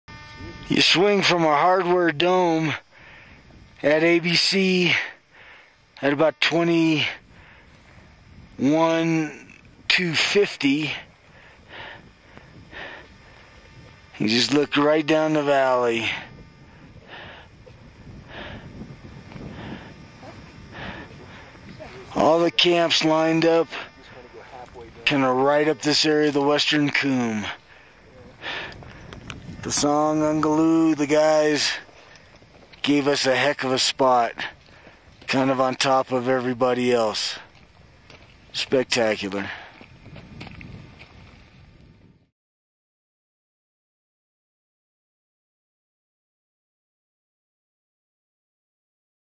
The Valley of Silence, or Western Cwm of Mount Everest with Top Leadership Speaker Susan Ershler
A quick look at our view from camp in 2001 straight down the Western Cwm, which people also call the Valley of Silence. You can't imagine how it feels to be there. Susan Ershler and her husband Phil Ershler set a world record by being the first couple to climb all Seven Summits together.